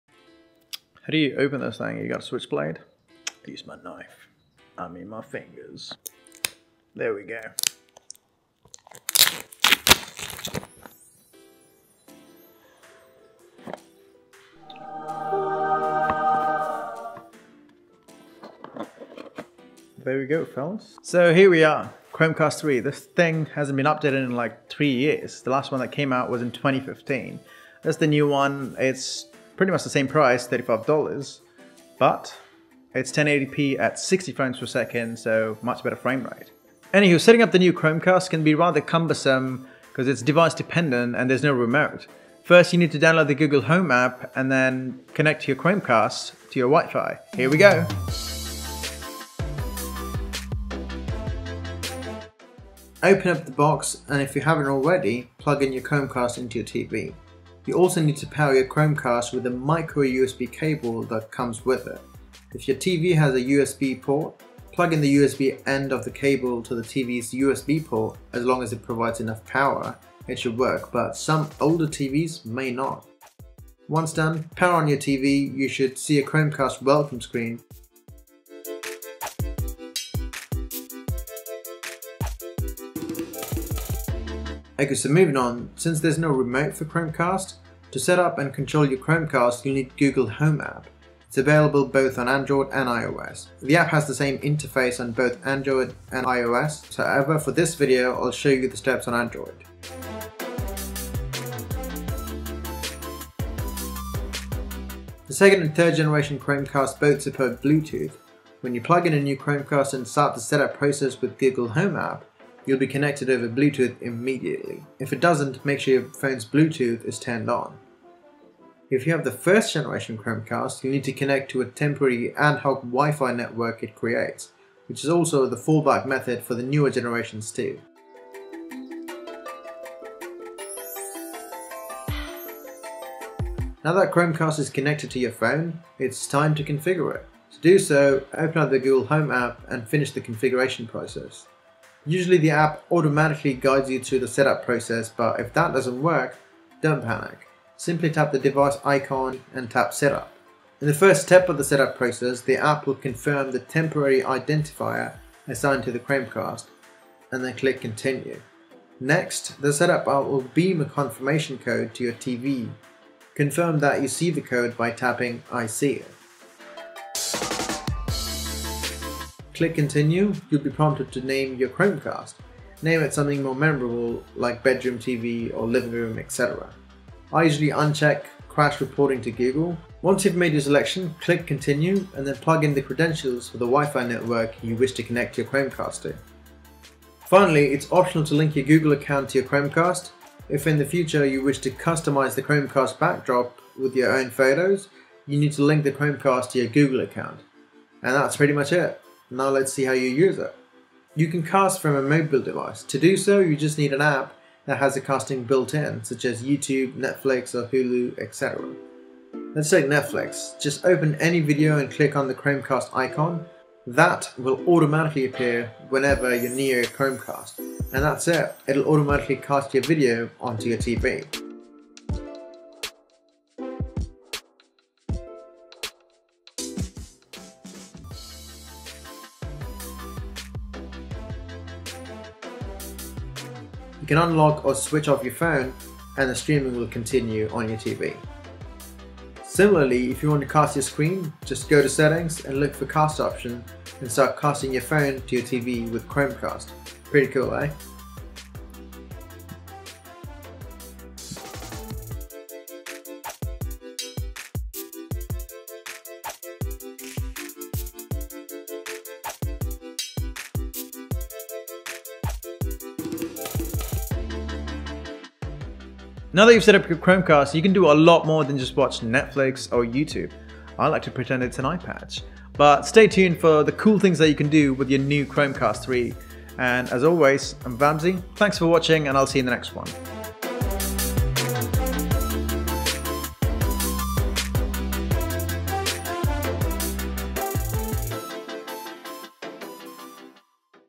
How to setup Chromecast 3
This video shows how to setup chromecast 3 (or even the older chromecast 2 and 1if you have it) on Android and iOS.